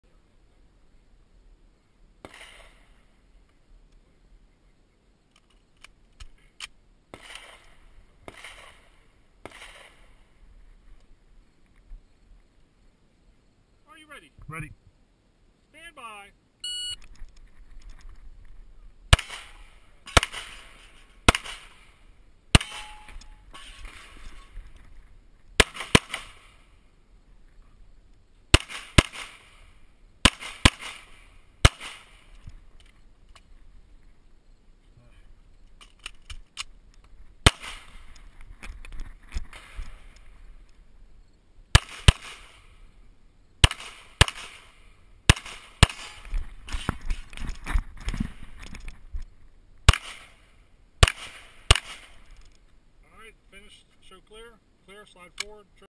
Bridgeville IDPA Stage 3 10/07/17
My first stage of my first match after about two years off.  Lots of rust.  Procedural penalties, clumsy the shooting order, and fumbling with the reloads.